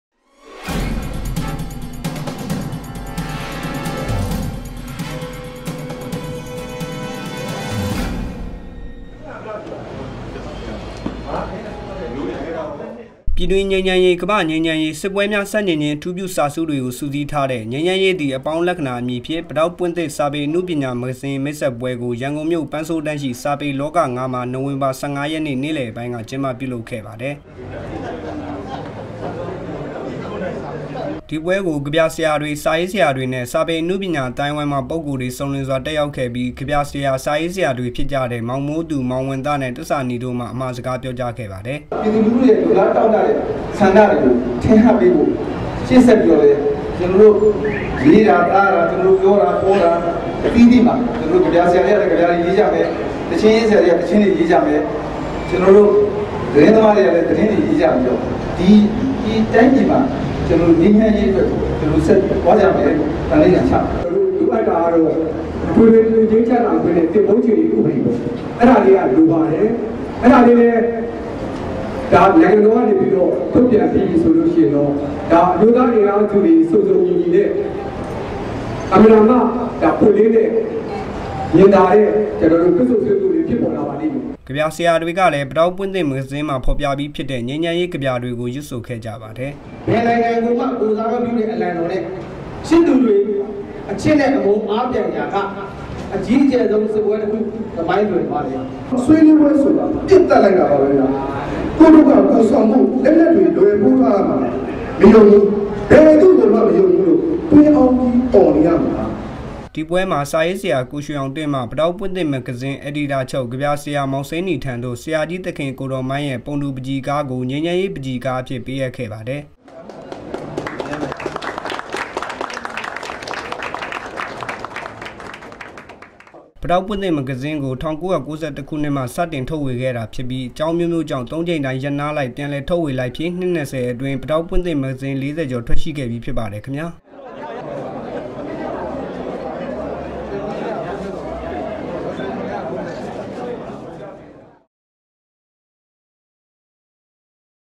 Press meeting of literature magazine,PadaukPwinThit in Yangon
ျပည္တြင္းျငိမ္းခ်မ္းေရး ၊ကမၻာျငိမ္းခ်မ္းေရး၊ စစ္ပဲြမ်ားဆန္္႔က်င္ျခင္း အထူျပဳစာစုေတြကုိ စုစညး္ထားတဲ့ ျငိမ္းခ်မ္းေရးသည္အေပါင္းလကၡဏာ အမည္ျဖင့္ ပိေတာက္ပြင့္သစ္ စာေပအႏုပညာမဂၢဇင္း မိတ္ဆက္ပဲြကုိ ရန္ကုန္ၿမိဳ႕ ပန္းဆိုးတန္း ရိွ စာေပေလာက ၅မွာ ႏိုဝင္ဘာ (၁၅)ရက္ေန႔ ေန႔လည္ပိုင္းက က်င္းပျပဳလုပ္ခဲ့ပါတယ္